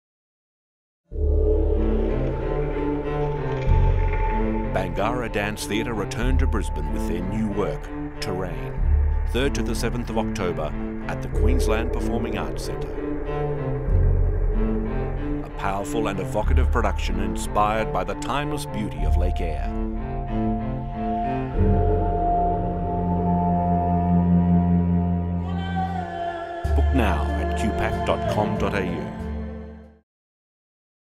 Bangarra Terrain (tv ad)
This is the television advertisement that I edited for the show Bangarra Terrain. The show was performed in QPAC in 2012 and it featured the dancer Ella Havelka, who went on to make history that year by becoming the first indigenous dancer to join the Australian Ballet.